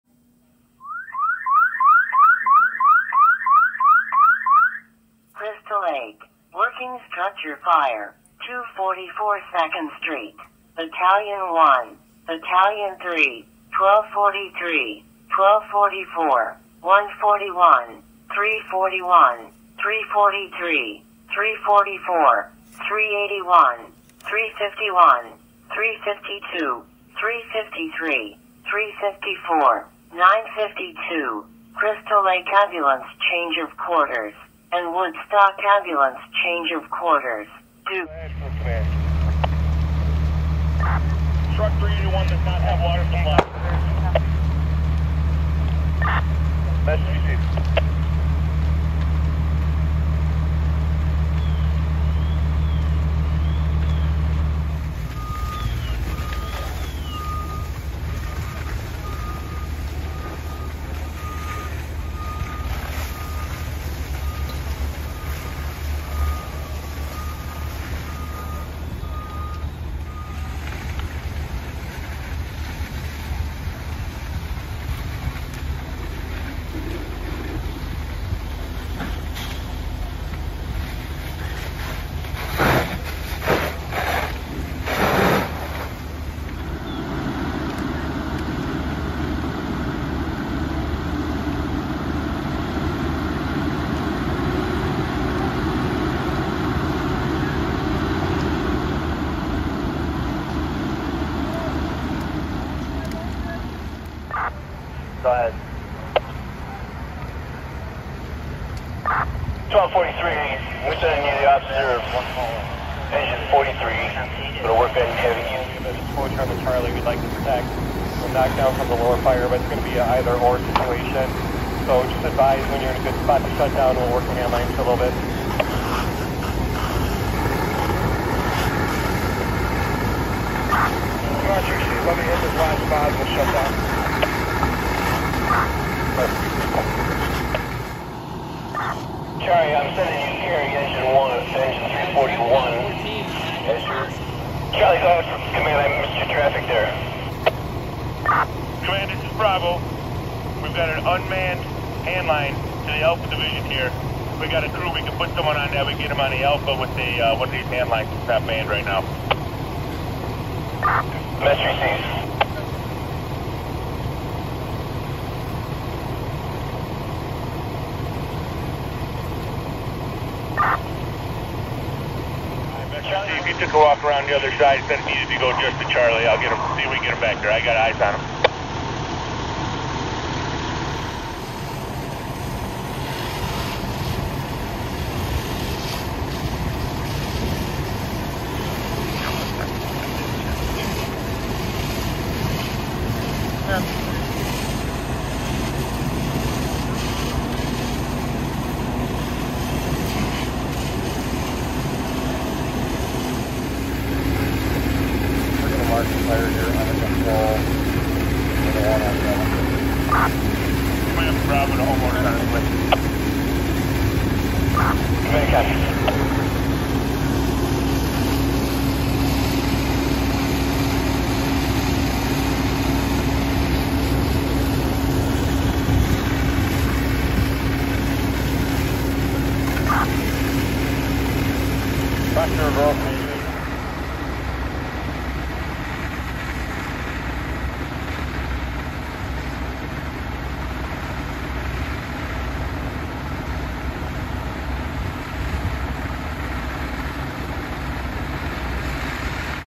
Crystal Lake (IL) Box Alarm House Fire (3/20/24)
Fully involved two story house on arrival